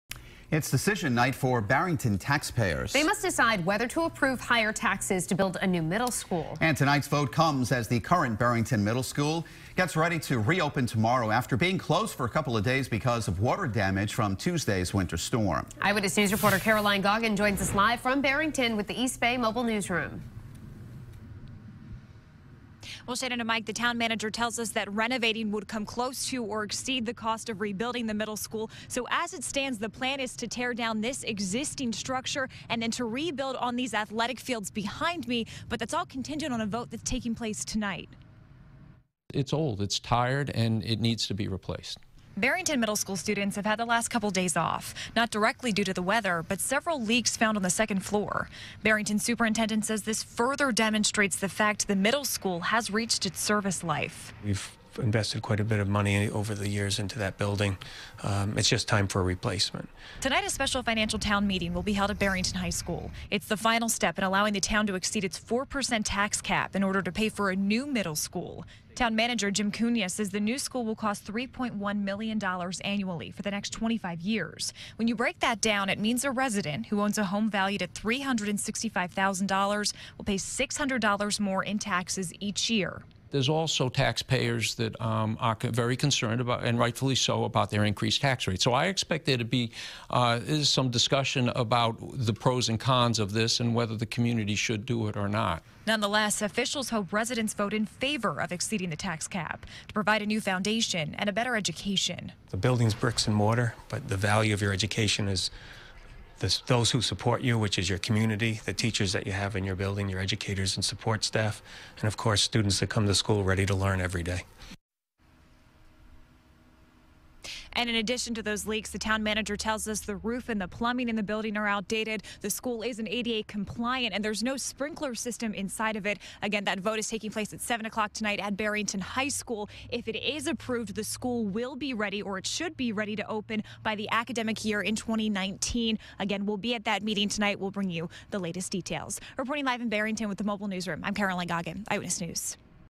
Town of Barrington Discusses New Middle School
According to the Town Manager Cunha, the school would cost $3.1 million annually for the next 25 years.